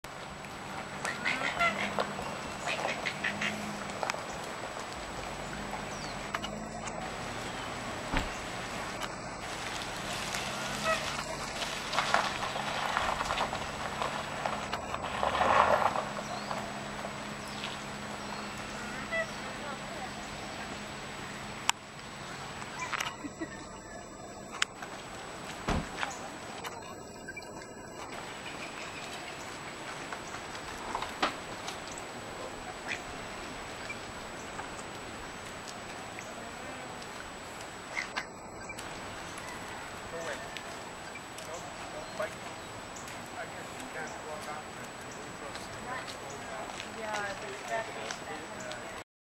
Nene (Hawaiian Goose), Kilauea Lighthouse, on the Garden Island of Kauai
Nene are rare Hawaiian geese that you tend only to find in mountainous parts of Hawaii, like Volcanoes National Park and Haleakala National Park.  These two were spotted strolling around the parking lot of the Kilauea Lighthouse on Kauai